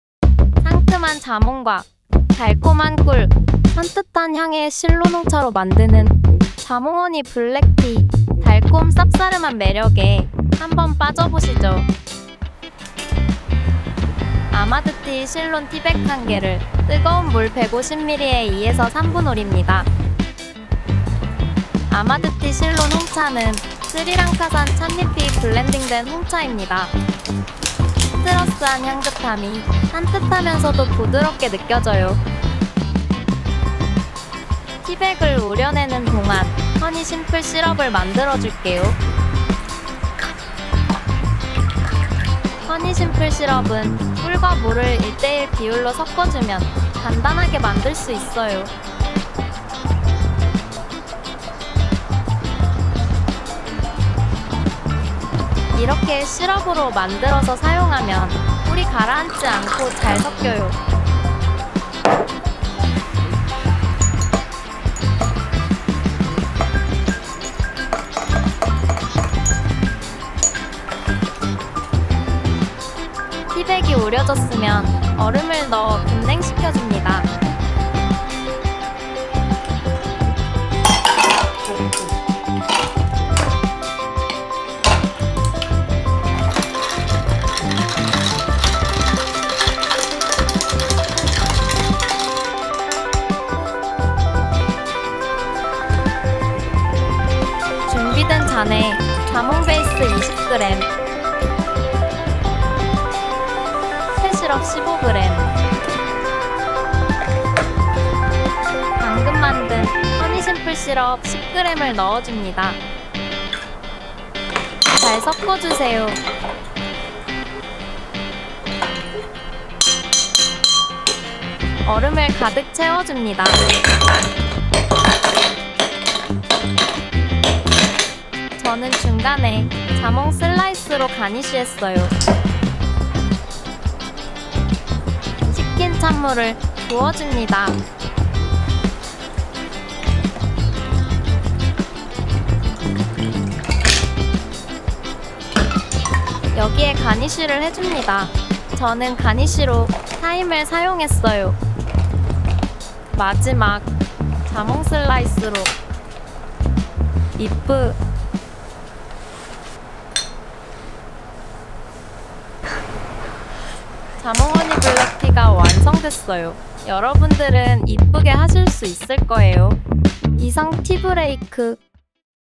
[AMAHD TEA] 상큼 달콤한 자몽허니블랙티
상큼 달콤한 자몽허니블랙티!!

매년 여름이면 생각나는 아이스티?!
상큼한 자몽과 달콤한 꿀, 실론 홍차를 넣어
산뜻한 향이 일품인 자몽허니블랙티는 달콤, 쌉싸름 하면서도
홍차의 향긋함이 느껴지는 시원한 아이스티 레시피입니다.

준비물

1. [아마드티] 실론 티백 1개
2. 뜨거운 물 150ml
3. [런던브릭스] 자몽베이스20g, 카페시럽 15ml
4. 허니심플 시럽 (꿀+물, 1:1비율) 10g
5. 가니쉬: 자몽 슬라이스, 로즈마리 등

만드는 방법

1. [아마드티] 실론 티백 1개 뜨거운 물 150ml 3분 우리기

2. [허니심플시럽] 꿀 1 : 물 1 의 비율 섞어주기

3. 자몽베이스20g 넣어준다

4. 카페시럽 15ml 넣어준다

5. 허니 심플시럽 10g 넣어준다

6. 얼음 170g 넣어준다

7. 찻물을 부어준다

8. 자몽 슬라이스, 로즈마리 등으로 가니쉬한다.